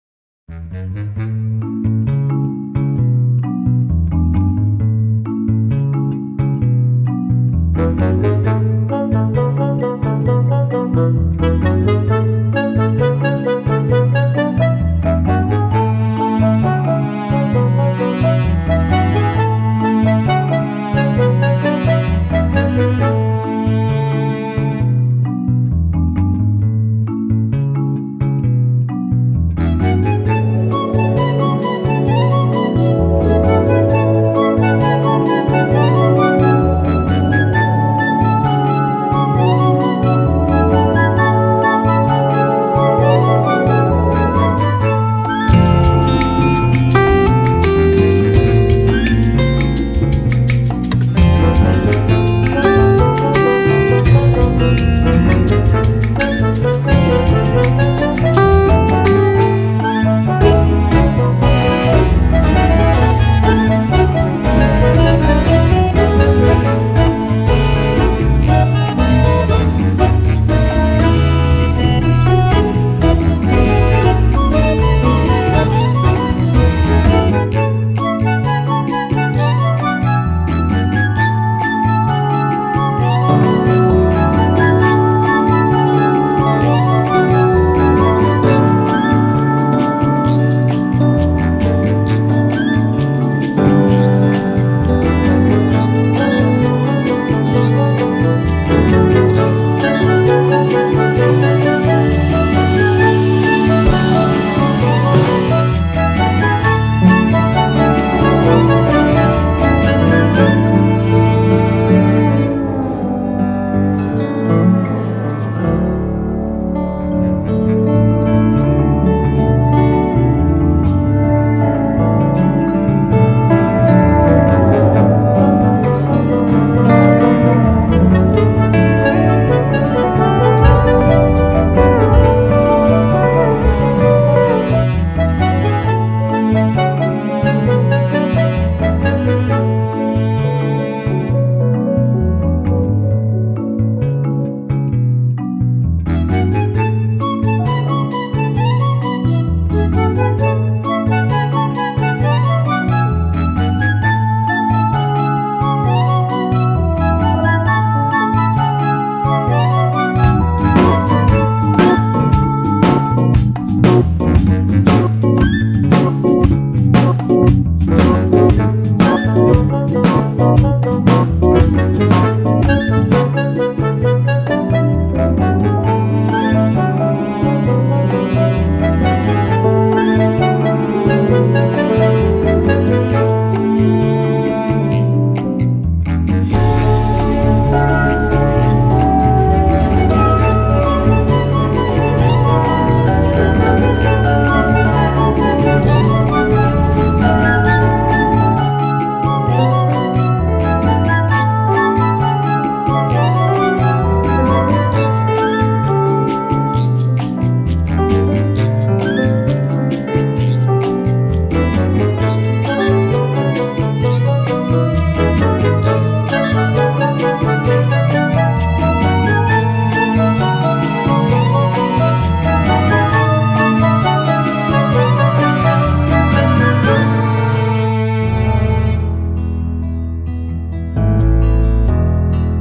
Cute Playing Funny kittens Compilation
This video is completely for the people who love pets specifically the people who love cats and thier cute act.